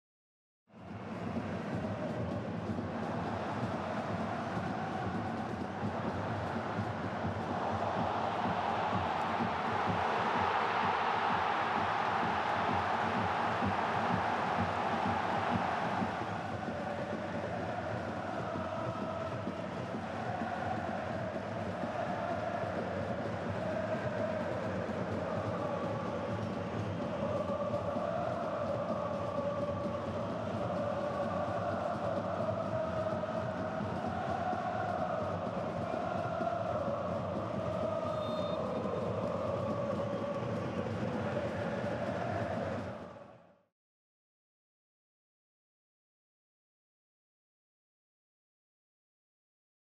J.League footballer scores amazing goal from halfway line
Urawa Red Diamonds' Thiago Santana scored an amazing long-range goal from near the halfway line in 24th minute of the team's J.League match against FC Tokyo at the Japan National Stadium in Tokyo.

Urawa Reds were ultimately beaten 2-1 but Thiago Santana's goal opened the scoring with excitement.